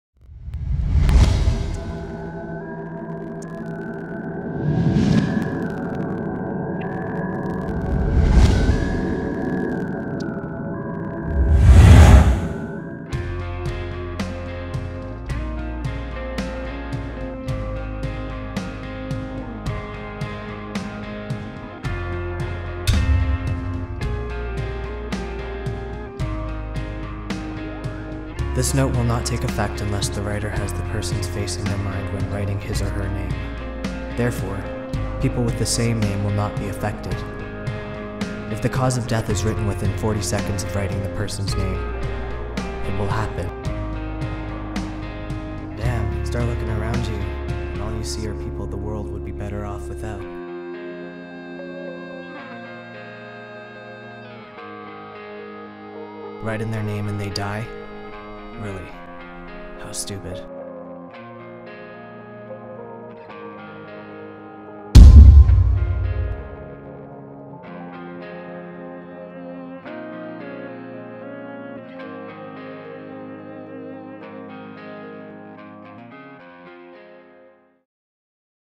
Death Note Teaser 2
This is another teaser trailer i made for my live action fan film Death Note. It is similar to the other one but i couldnt decide which one i like better.  If you like the trailer please check out the film it is in 2 parts.